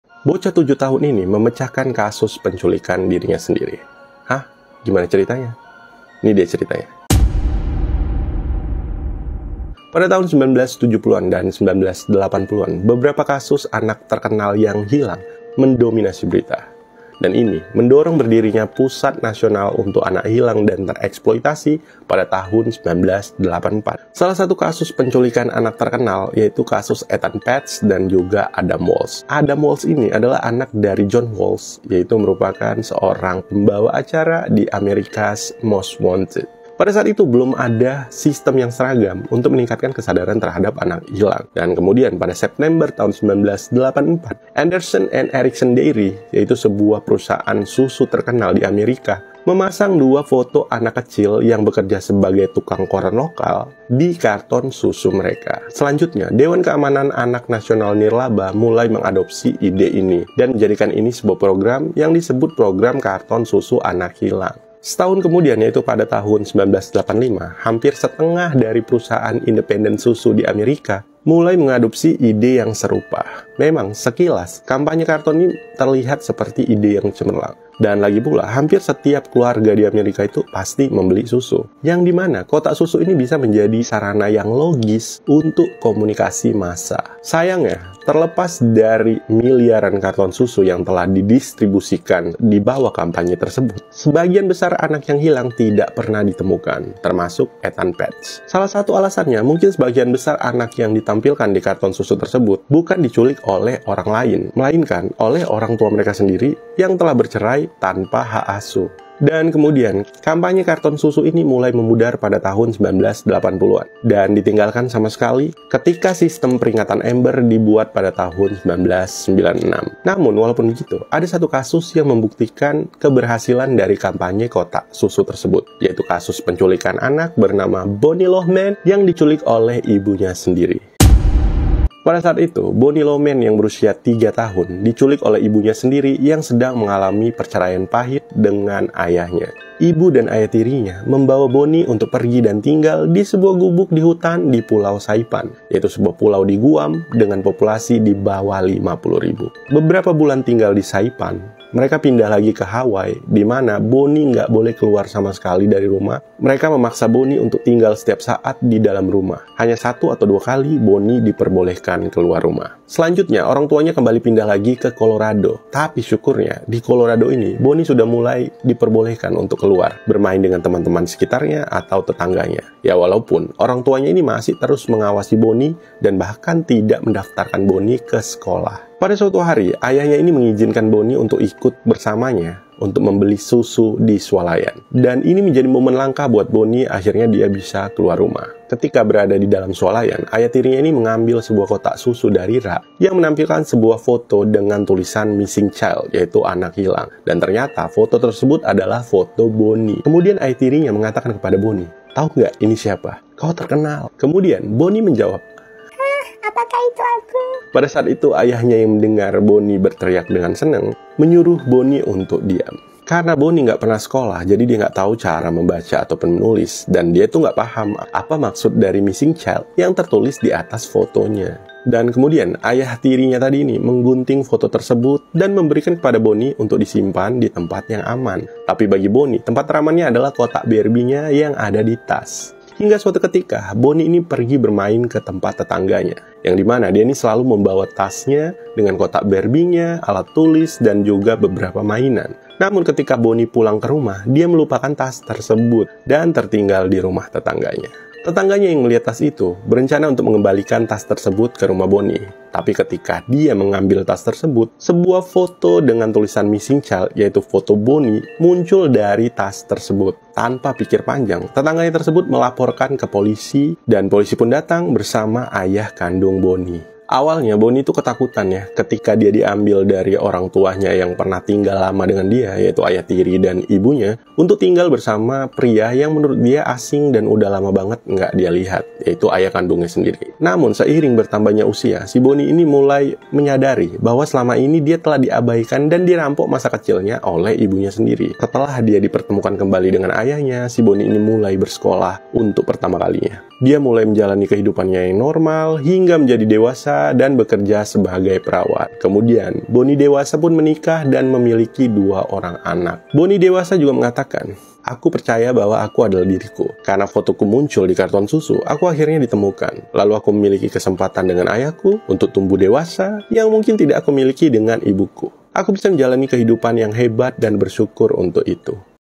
KAMPANYE KOTAK SUSU ANAK HILANG MEMBUATNYA MEMECAHKAN KASUS PENCULIKANNYA- BONNIE LOHMAN
Halo guys!! Makasih udah mau klik video ini.
Cerita kali ini tentang kampanye kotak susu anak hilang dan Bonnie Lohman

Terima kasih
Fabulous Team